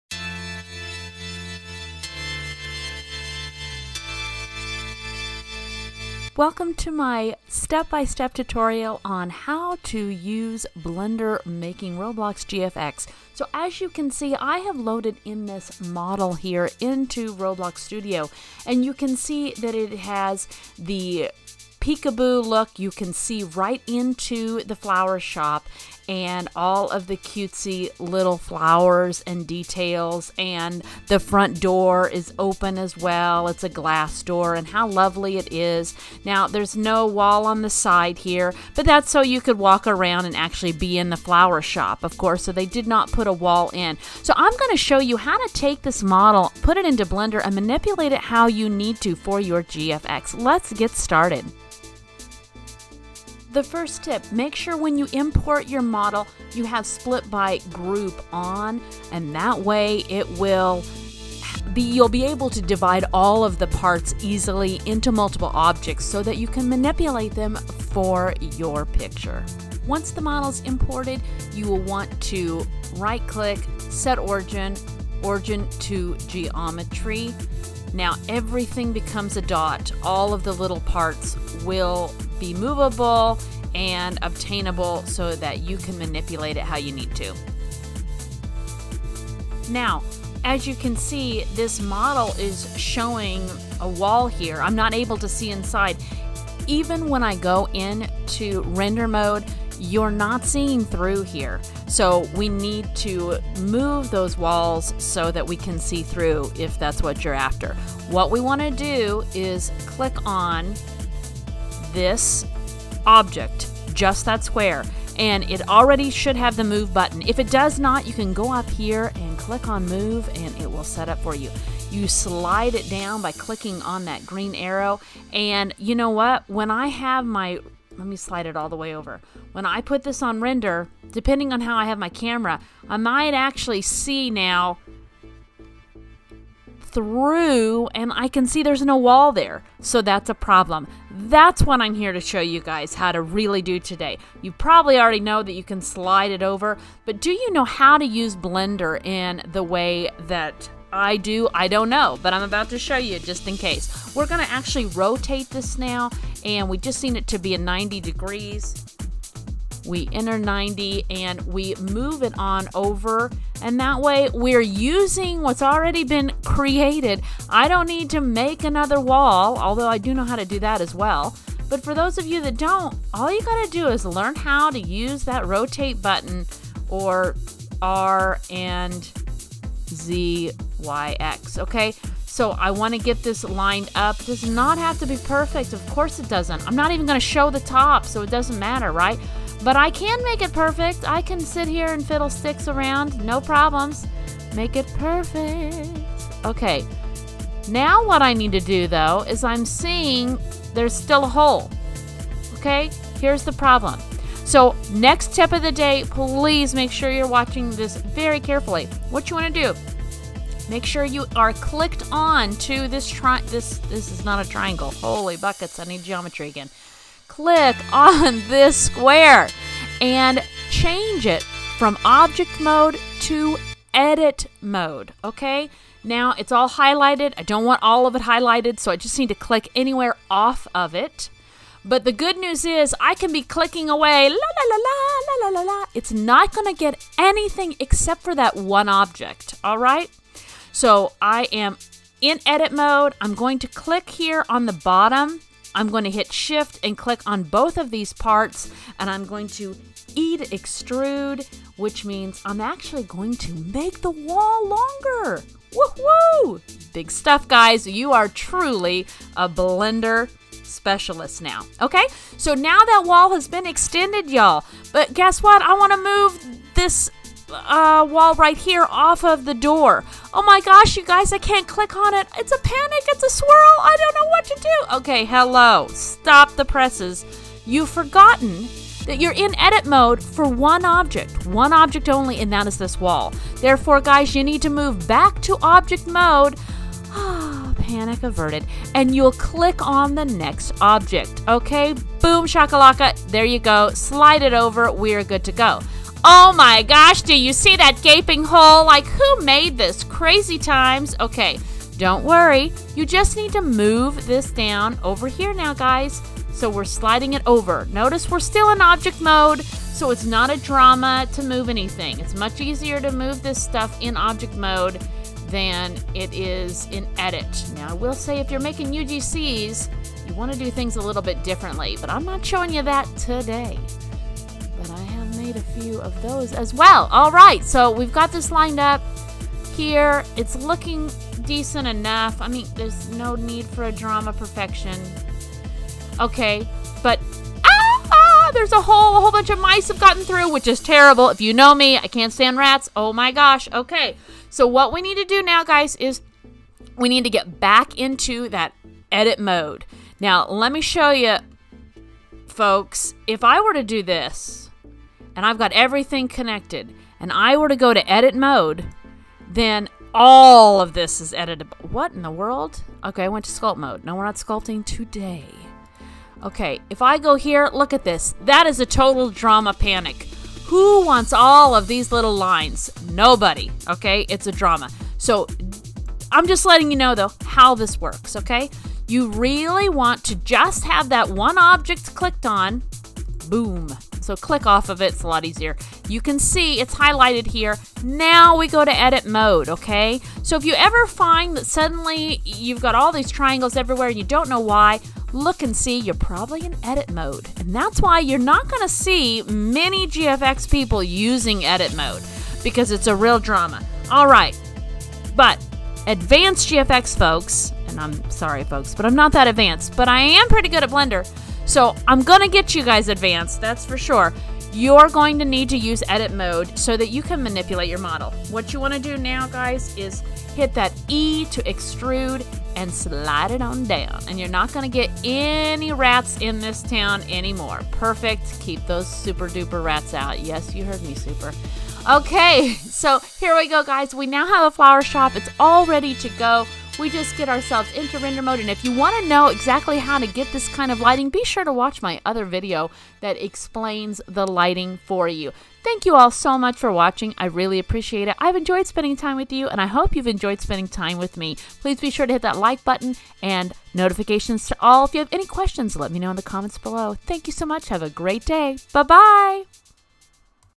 How to Edit Roblox Models In Blender | Building in Blender | Roblox GFX Models in Blender
Please enjoy this video on how to build and edit Roblox models using Blender!